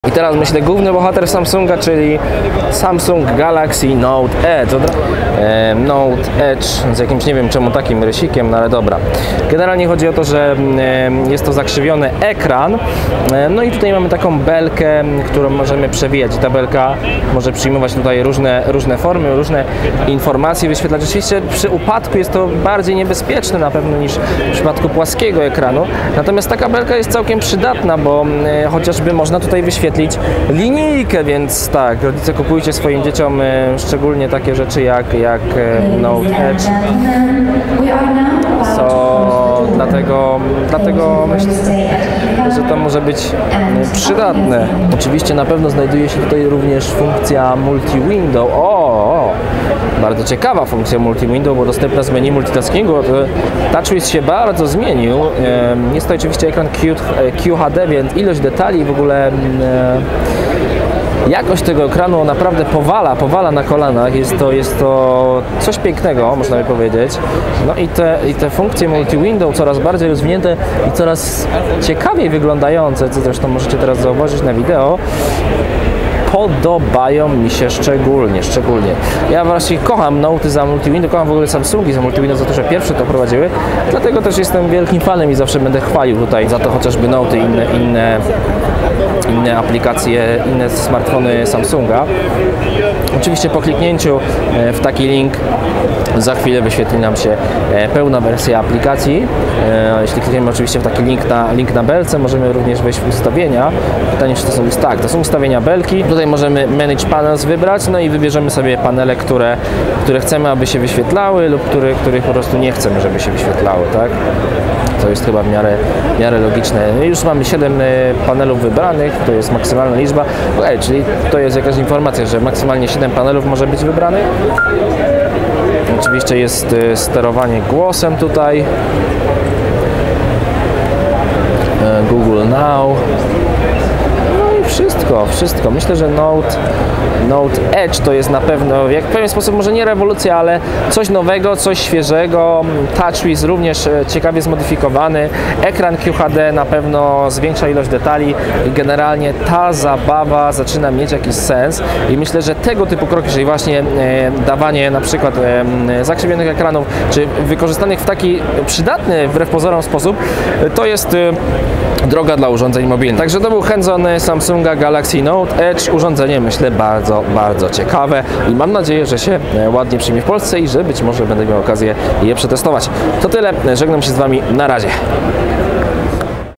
Samsung Galaxy Note EDGE - bardzo nowoczesny smartfon z IFA 2014
Samsung Galaxy Note EDGE został zaprezentowany na targach IFA 2014. Oryginalna funkcjonalność paska menu z boku ekranu ekrany jest jego największą zaletą. Zobacz nasz hands on Samsung Galaxy Note edge.

Redakcja Portalu
VideoTesty.pl